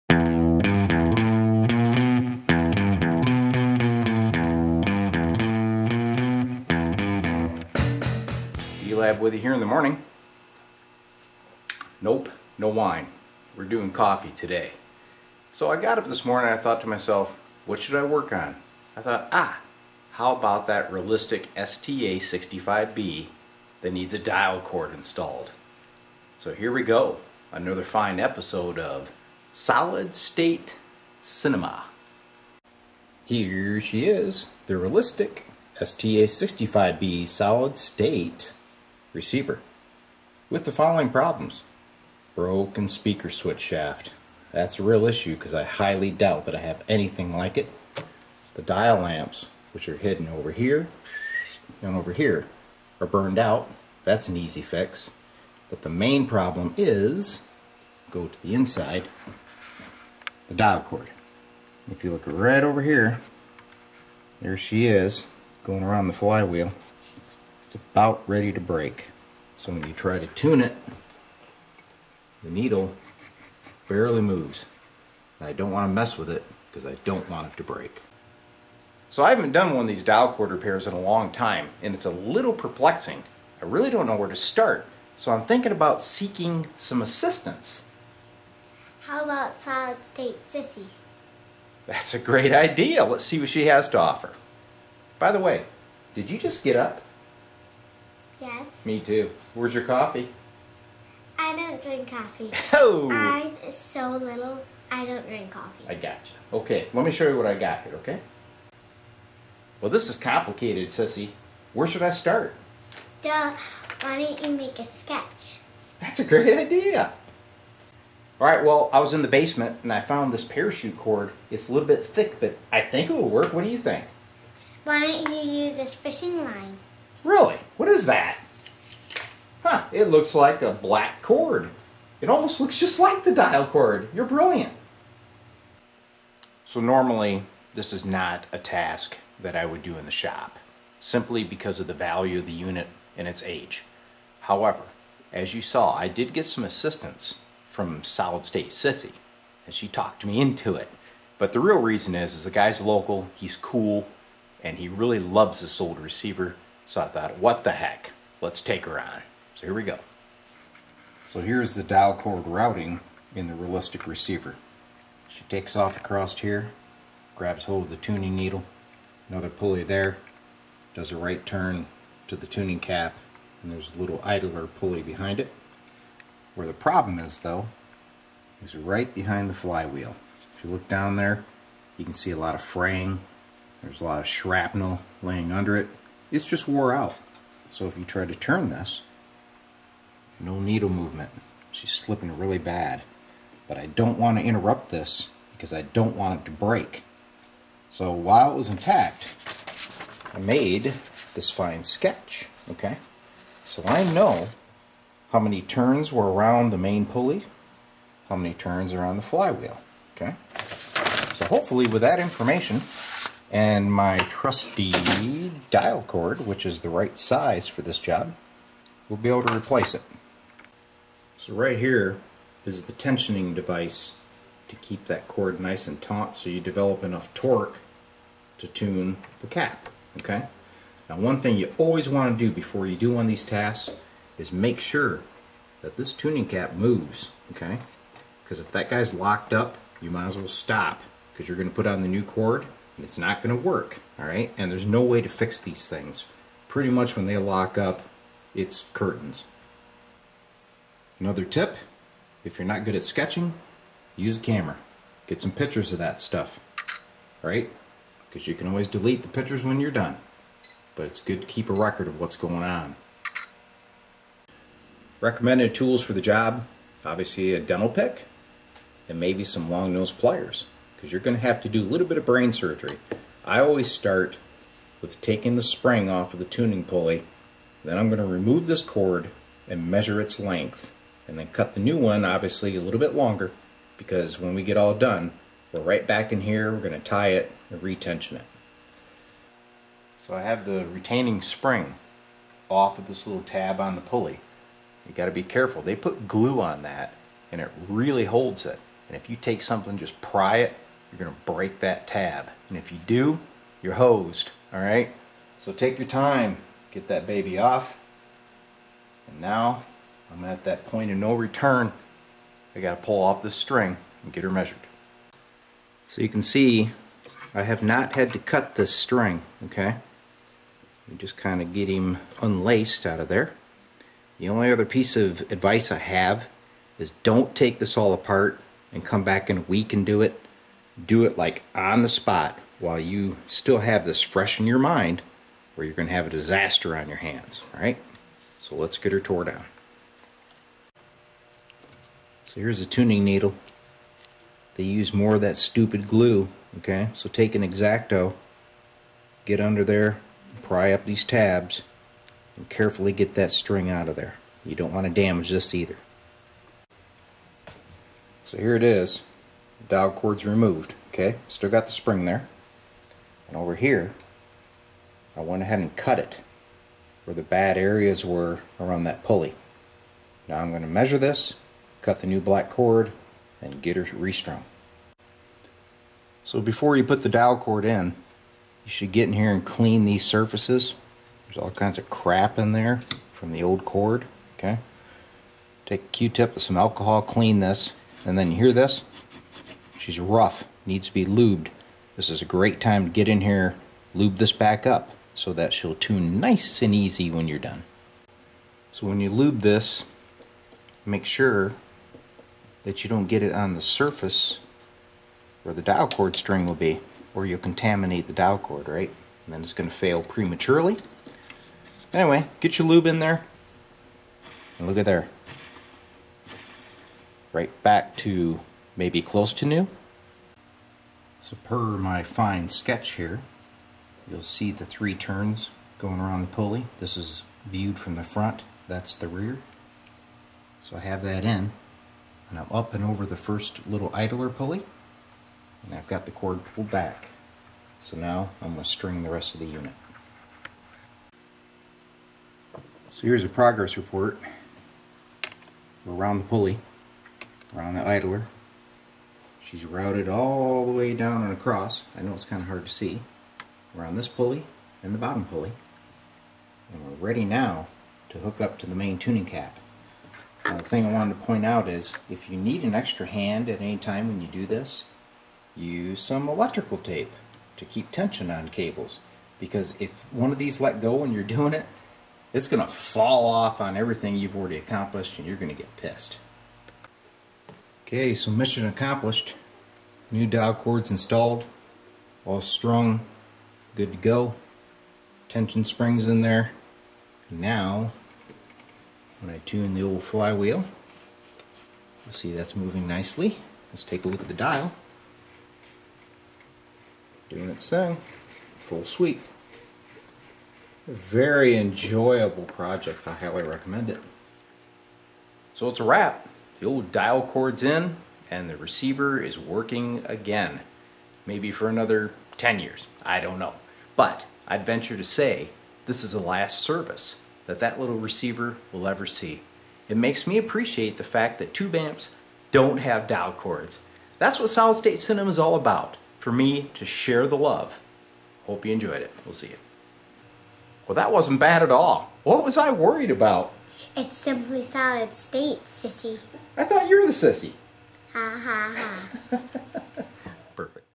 How to replace repair dial tuning cord vintage AM FM stereo receivers D-lab Electronics
This relic showed up at the shop by a local customer. Normally I would not perform this task due to the time involved and the value, or lack there of the receiver. However, the guy loved this old gal, so I caved in. It was not a bad task, but did take about 2 hours. Turned out well. Back in the day, I loved working on stereos, but for now, will stick to tube amps. Solid state Sinema with Sissy!